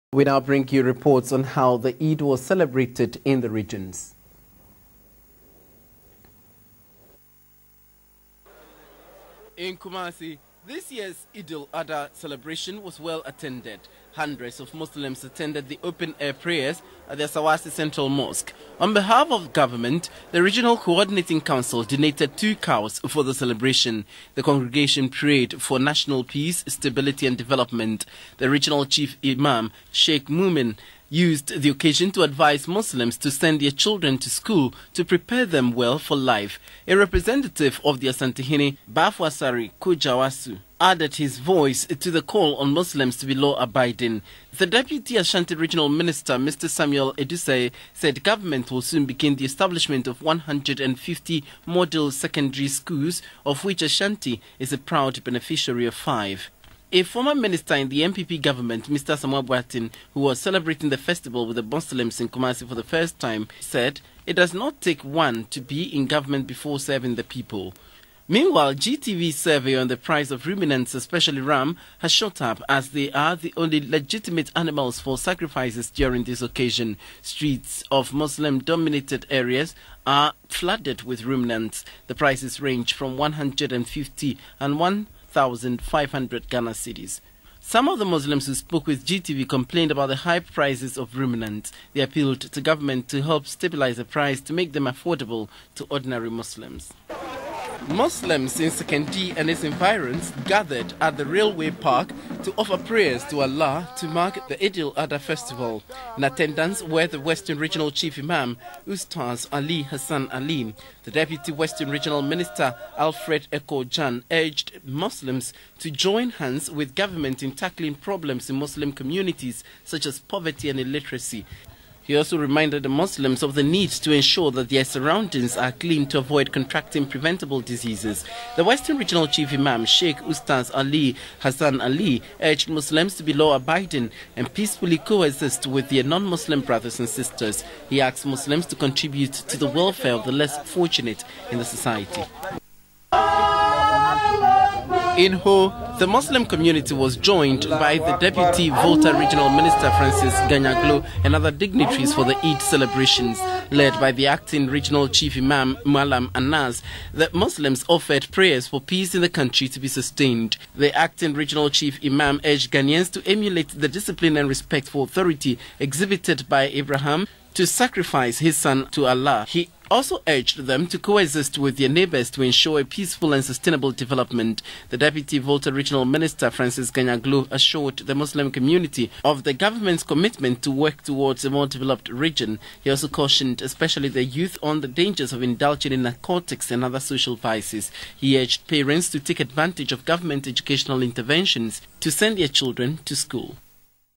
EID Celebrations In Other Regions
Regions:
We now bring you reports on how the Eid was celebrated in the regions